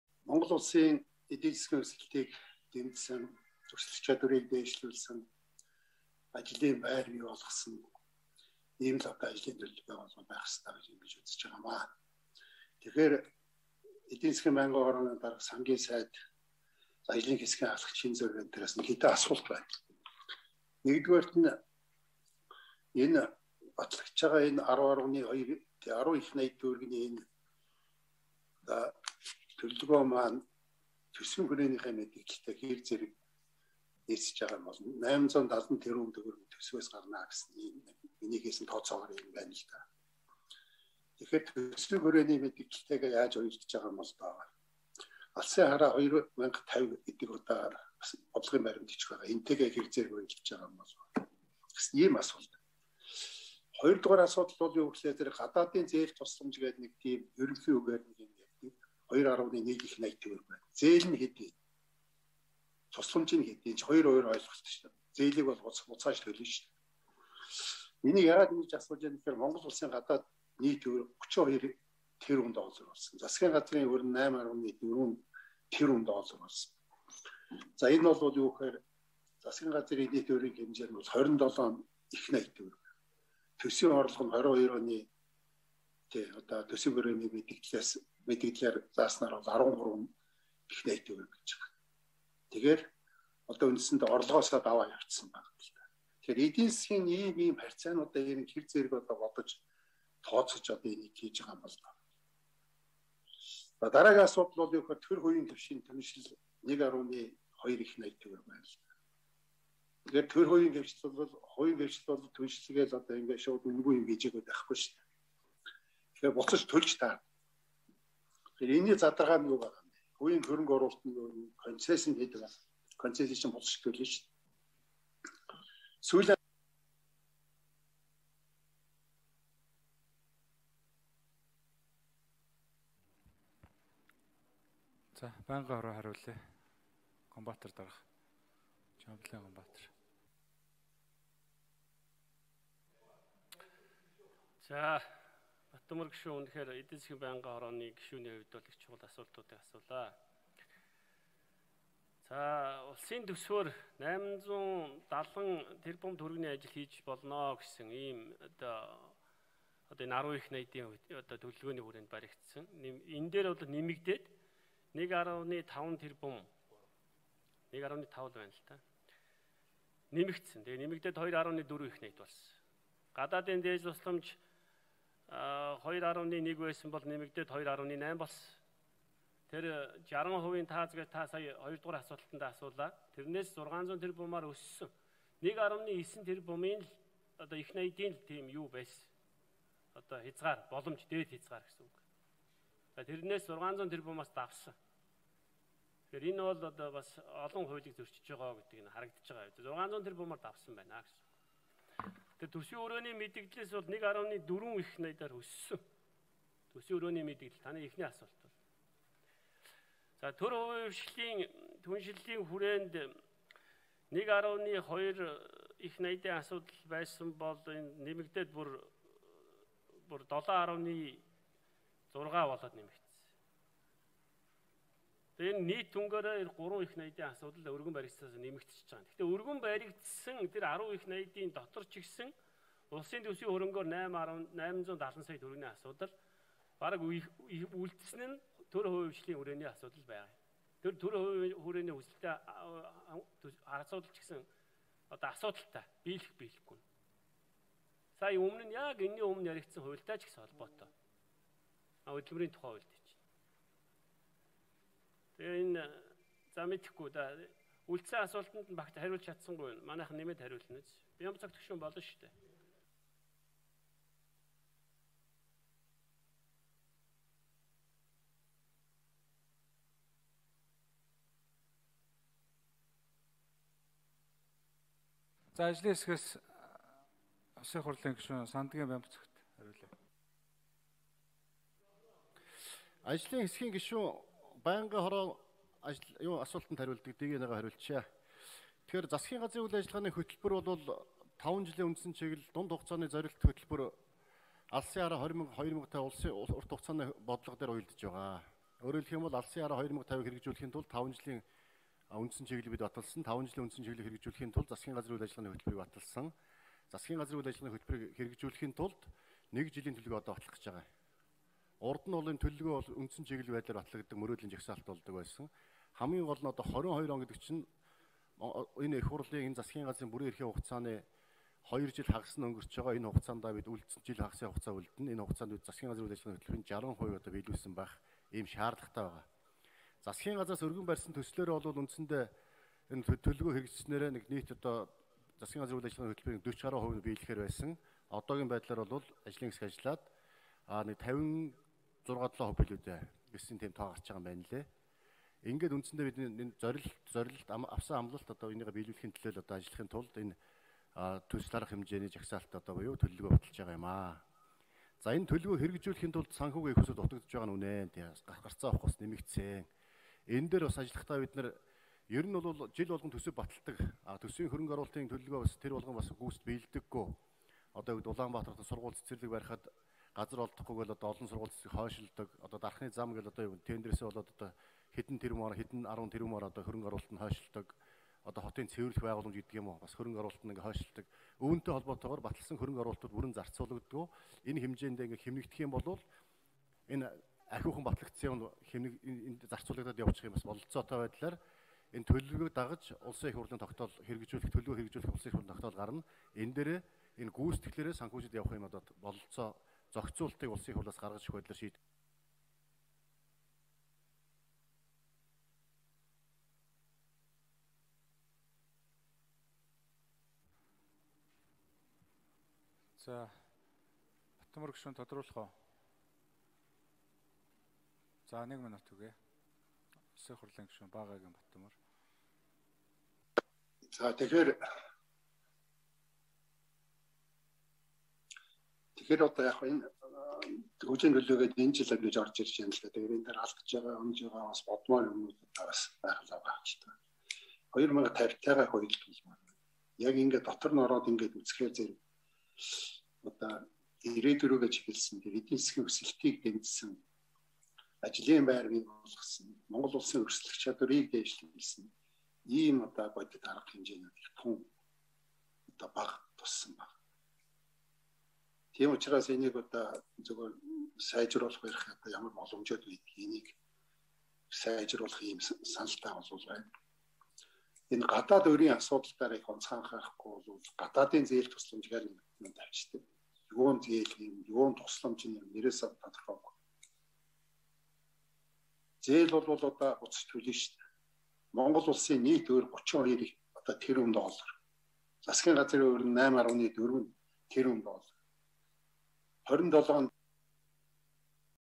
Б.Баттөмөр: Алсын хараа 2050 хөтөлбөр болон төсвийн хүрээний мэдэгдэлтэй Хөгжлийн төлөвлөгөө нь
Б.Баттөмөр: Алсын хараа 2050 хөтөлбөр болон төсвийн хүрээний мэдэгдэлтэй Хөгжлийн төлөвлөгөө нь хэрхэн уялдаж байгаа вэ?